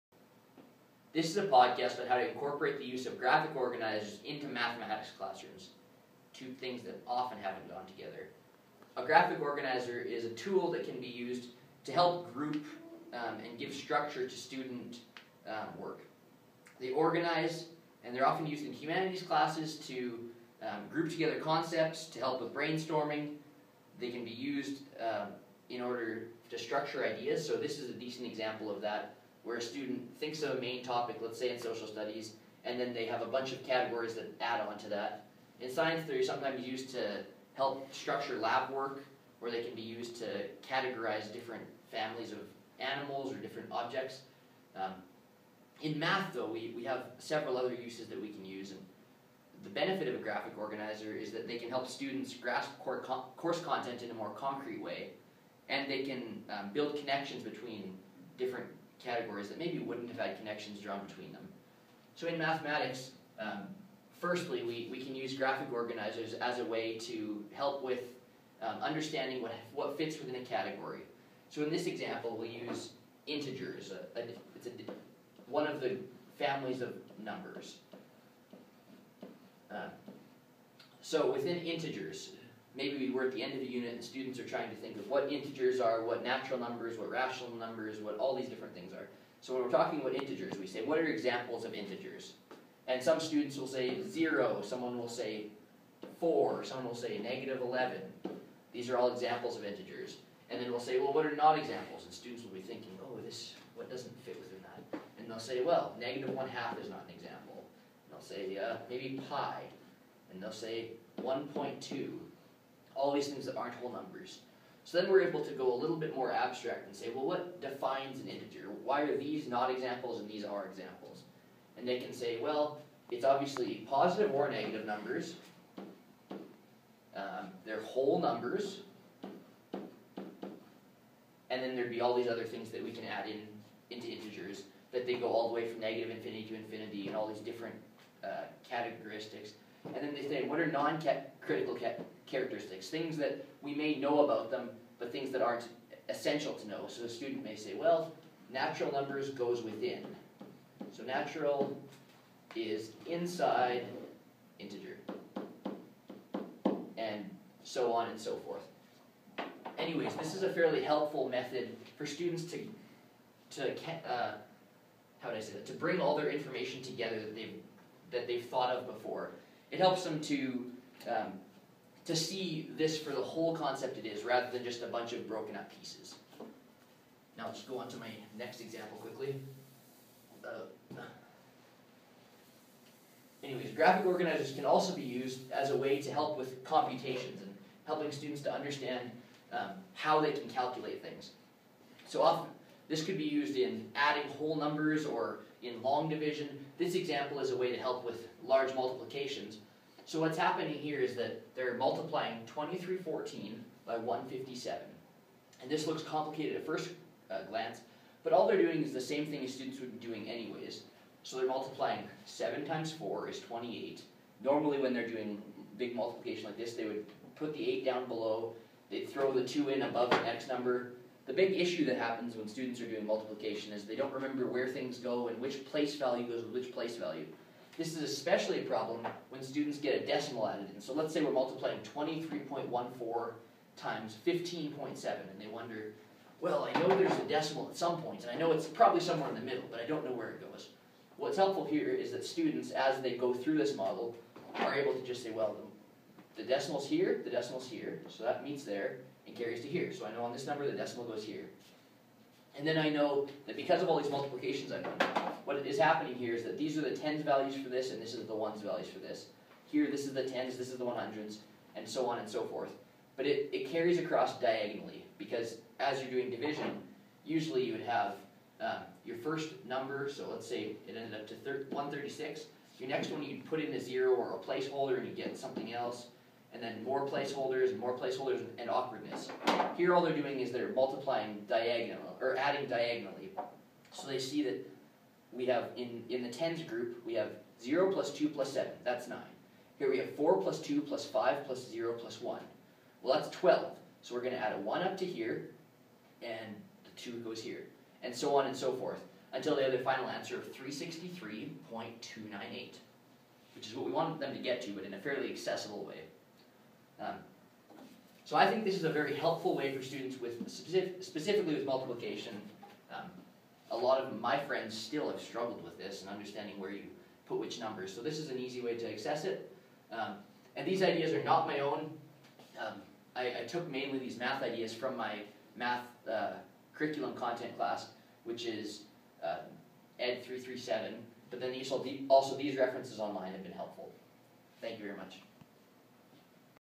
Using Graphic Organizers in Math
Here are some ideas about how graphic organizers can help students grasp mathematical concepts.